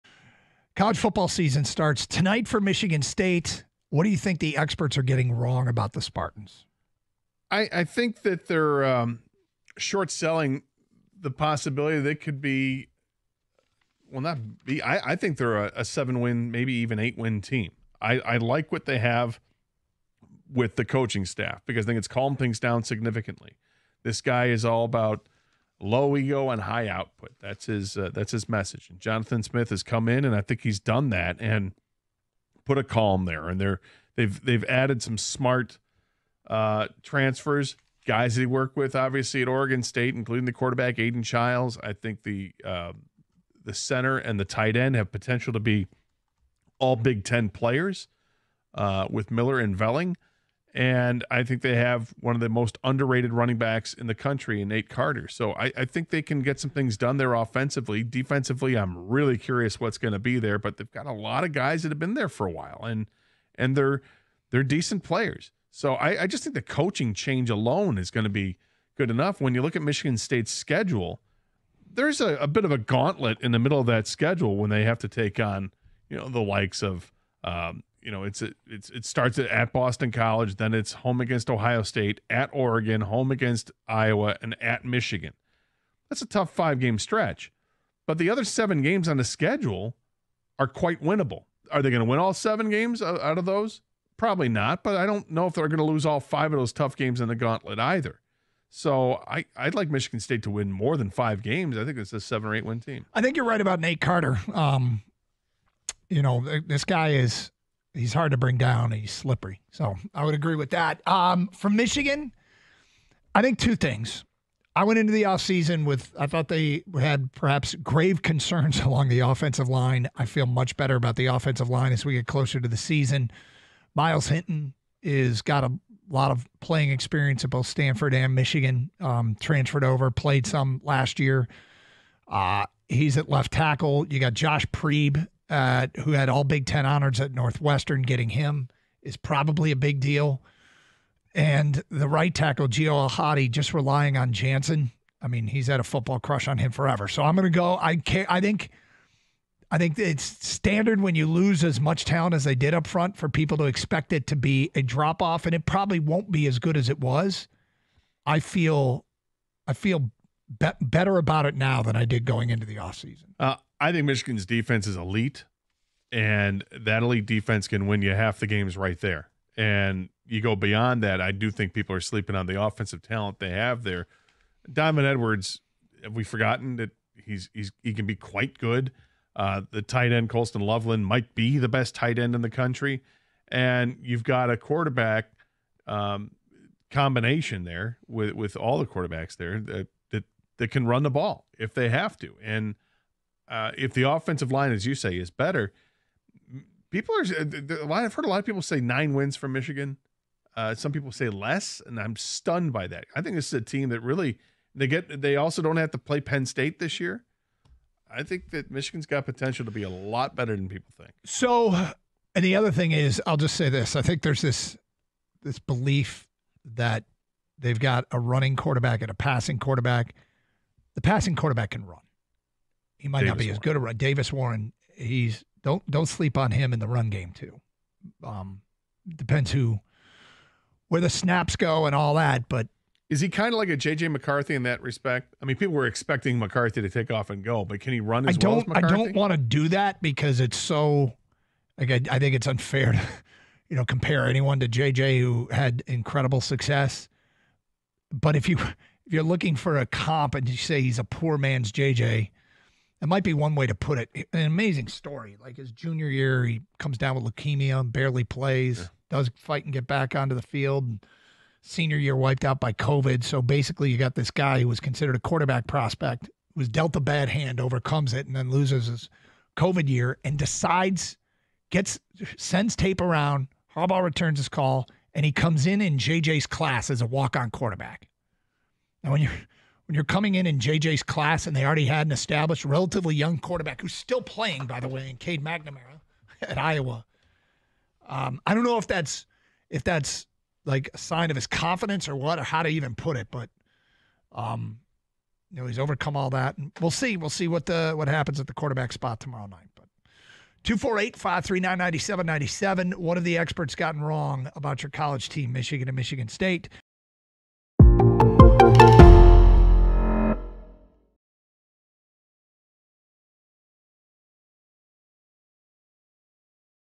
What Experts Are Getting Wrong About MSU & UofM | Karsch and Anderson
Doug and Gator have bones to pick with some of the pundit rankings.